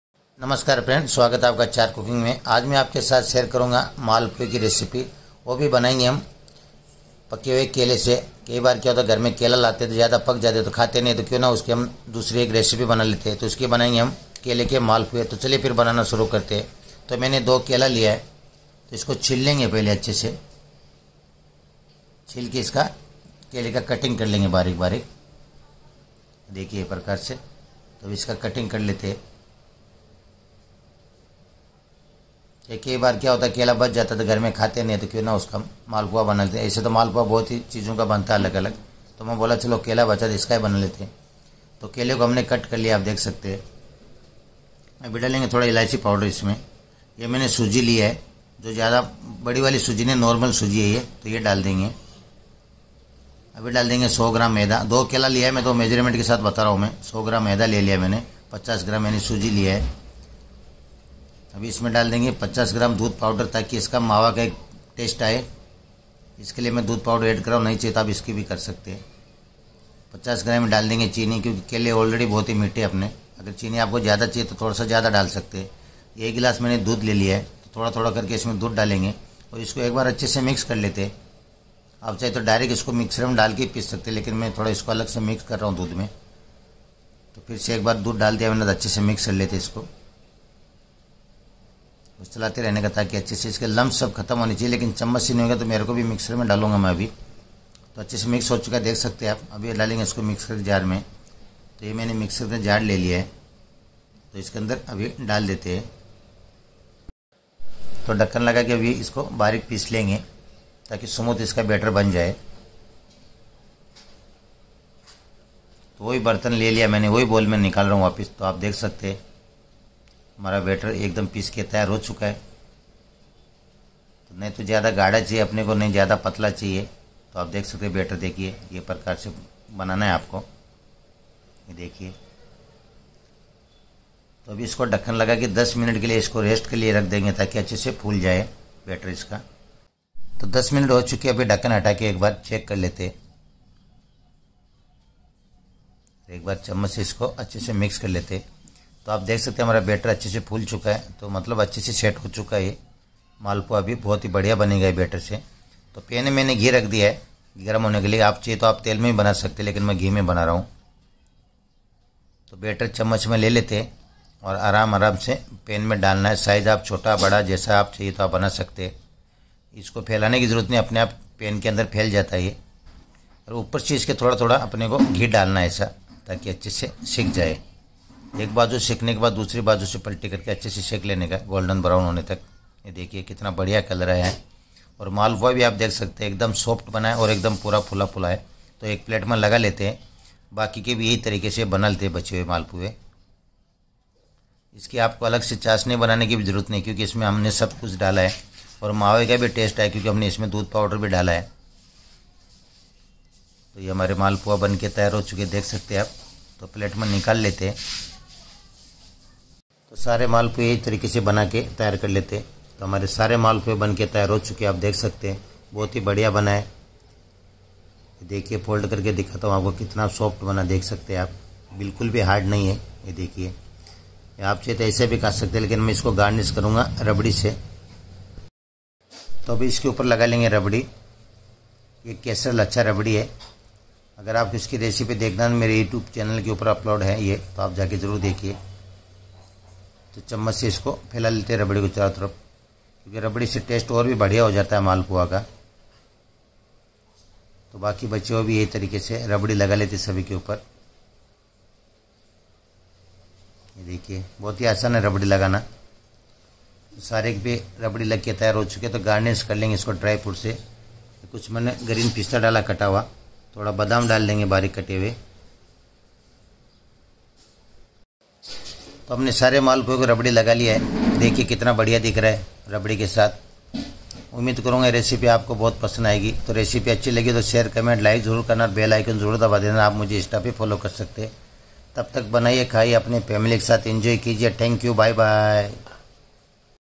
बिना चाशनी बिना किसी परेशानी के केले से बना मालपुआ Rabdi Malpua Recipe
Hi friends Swagat Hai Apka chahar Cooking
Aaj Main Apke Sath Share Karunga Navratri Rabdi Malpua Recipe